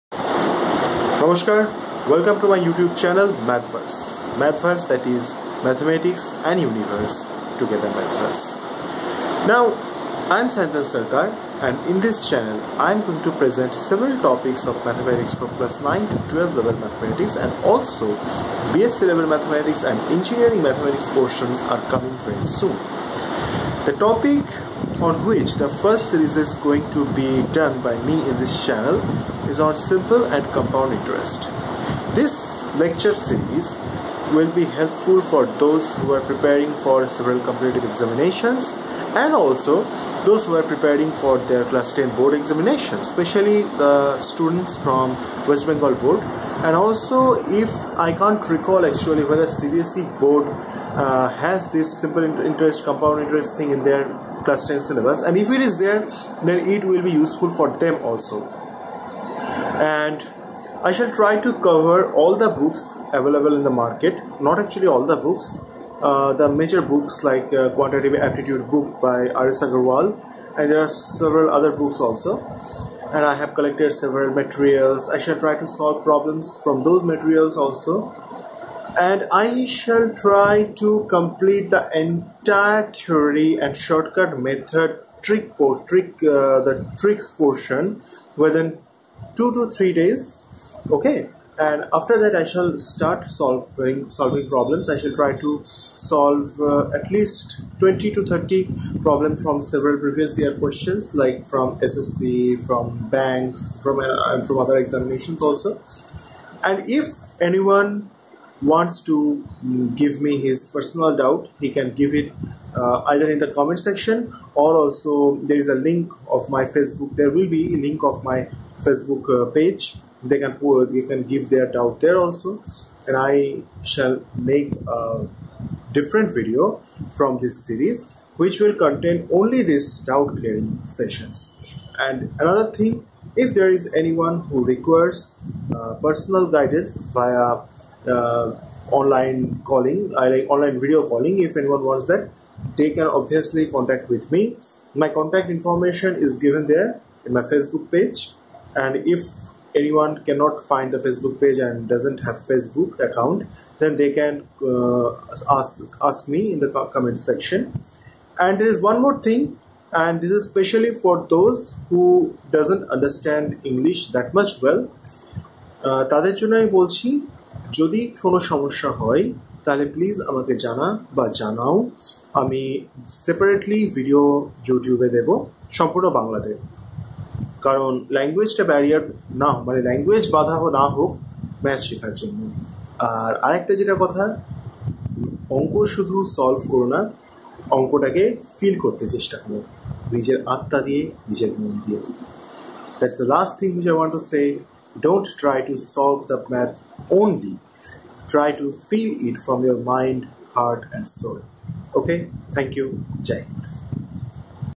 Simple Interest- Introduction
This is the first video of the lecture series on Simple and Compound interest for competitive exams and board exams.